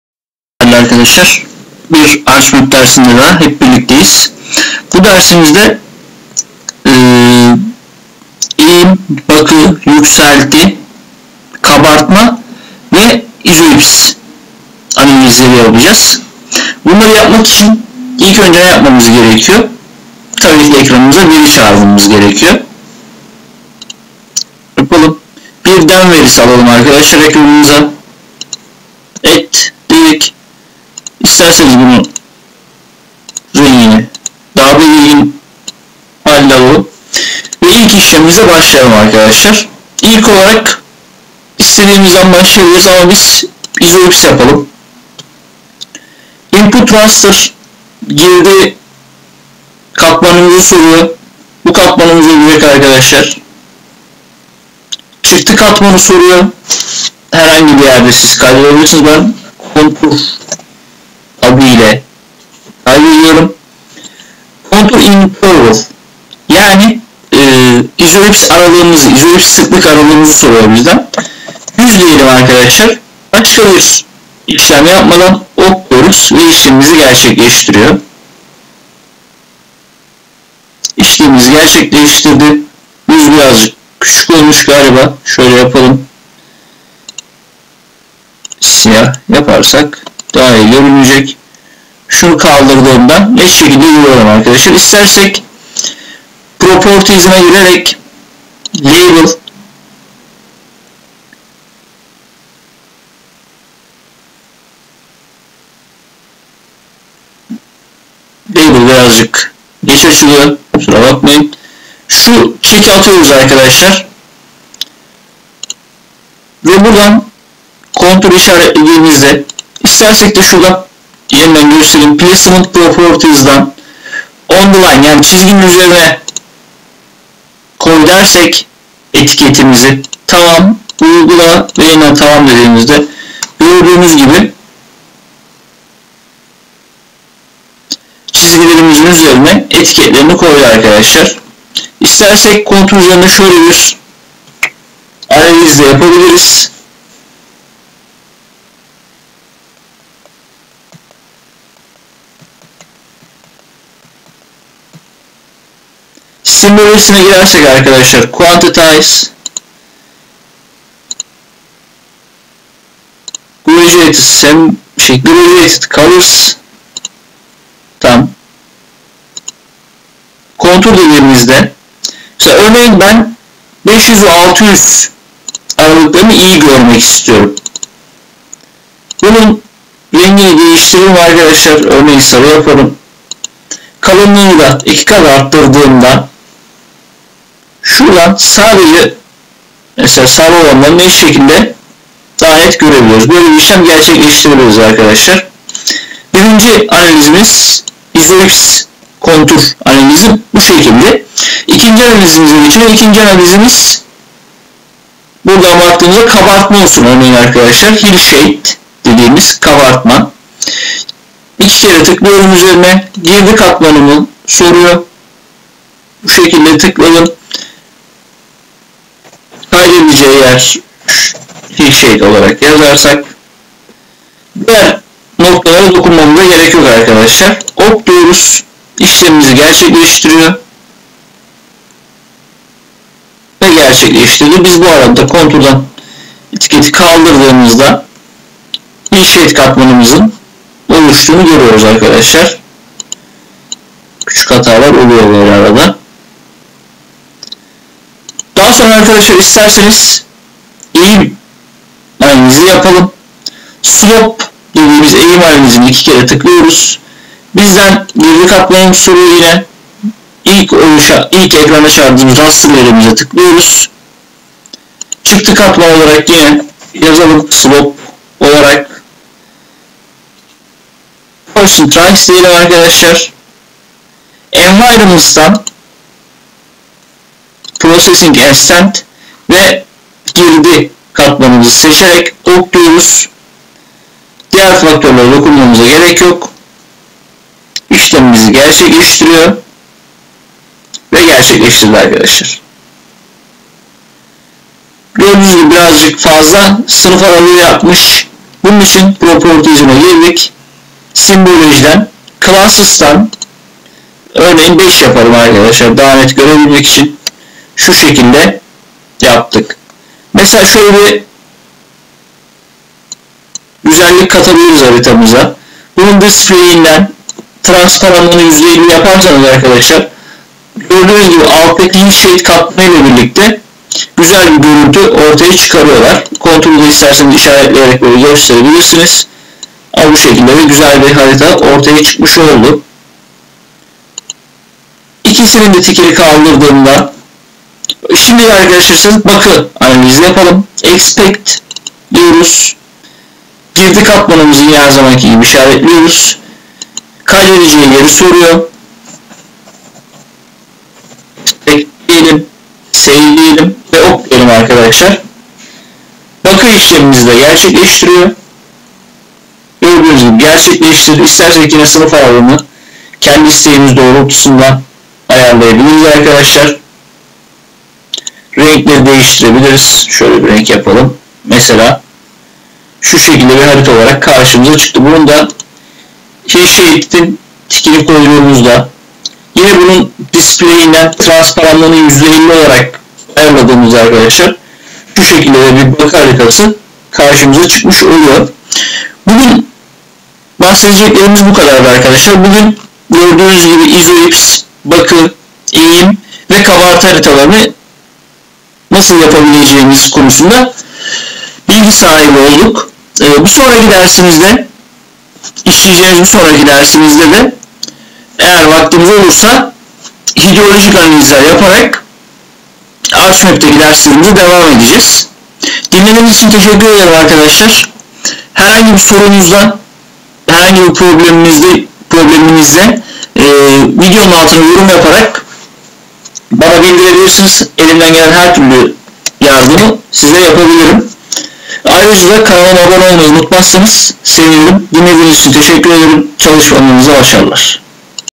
Arcgis-İzohips(Contour)Eğim(Slope) Bakı(Aspect) Kabartma(Hillshade) Haritaları Yapımı
Argis'de eğim, bakı, yükselti, kabartma analizleri yapımı...